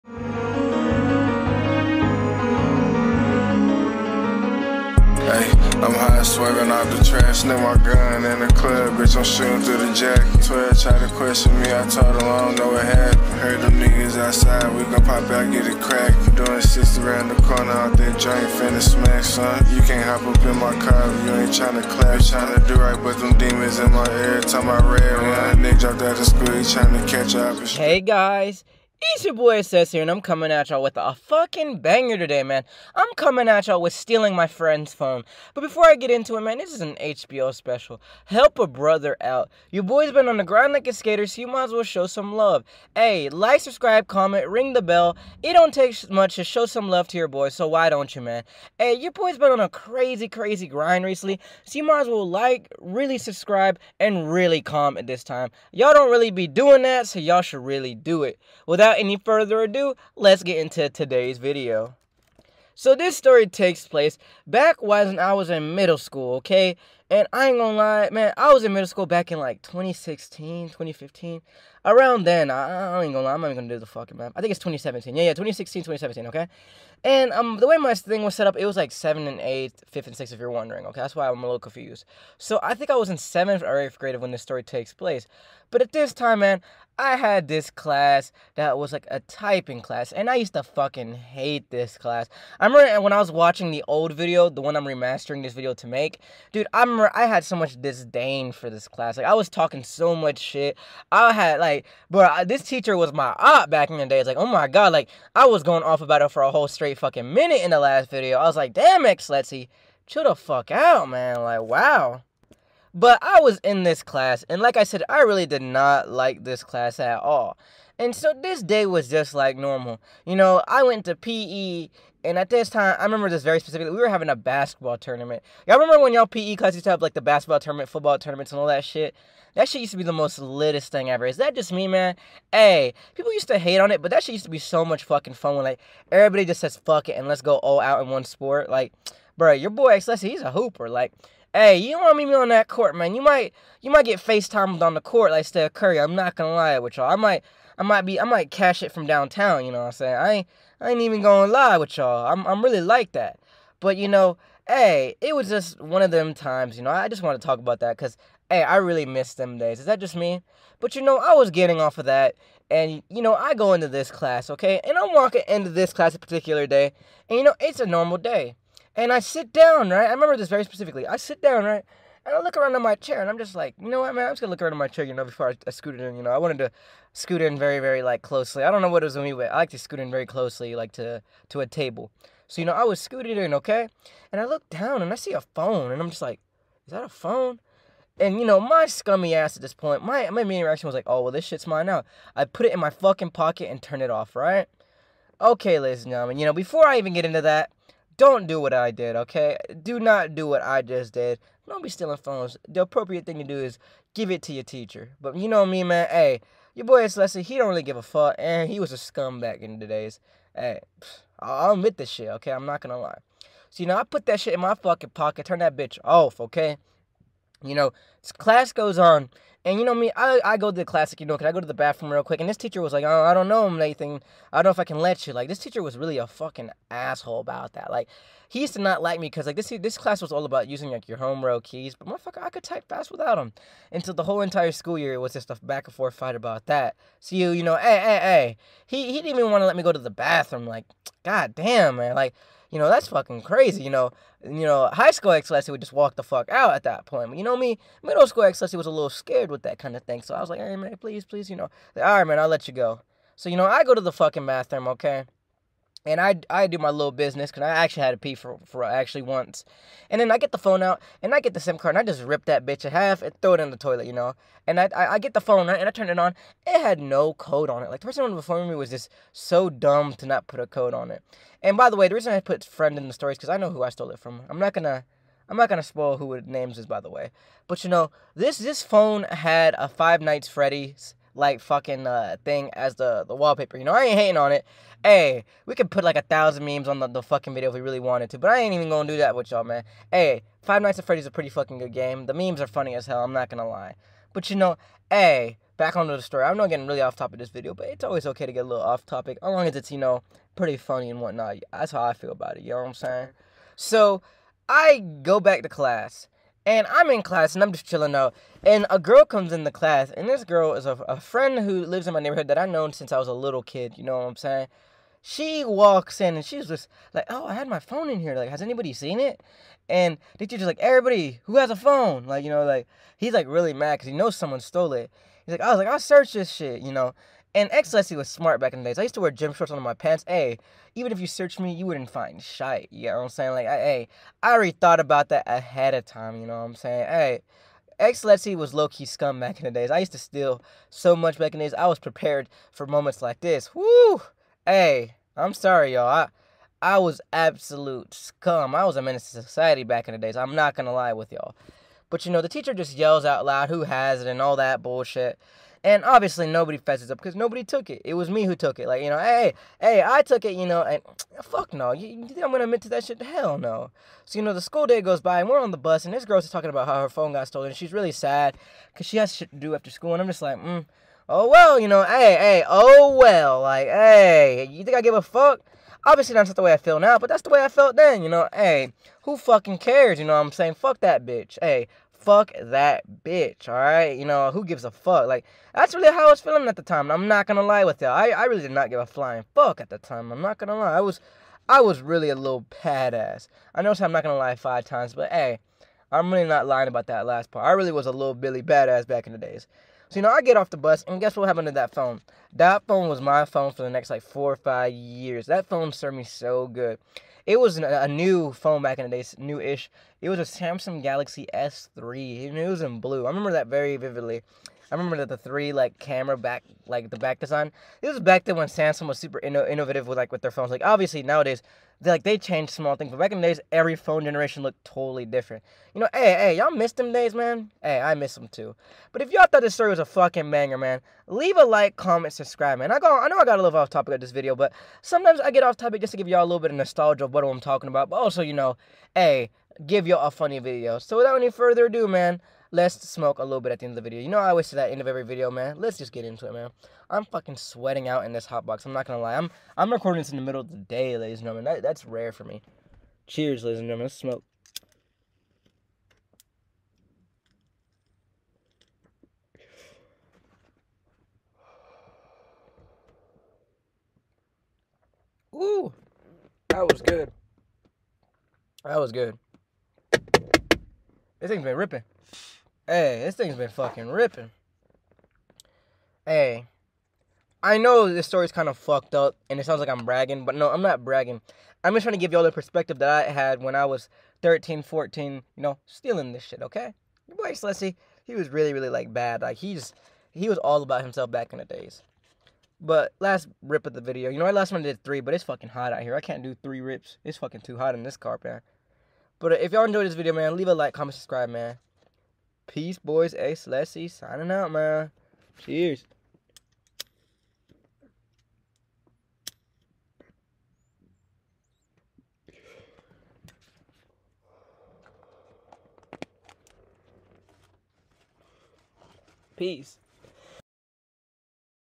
STEALING A PHONE AT SCHOOL!?! (MY FRIEND'S?!) (CRAZY LIFE STORY)
I mean dropping crazy for y'all so I can beat the YouTube shadow! this ia a banger story with good commentary too!  You guys are the best I really love dropping videos for y'all !!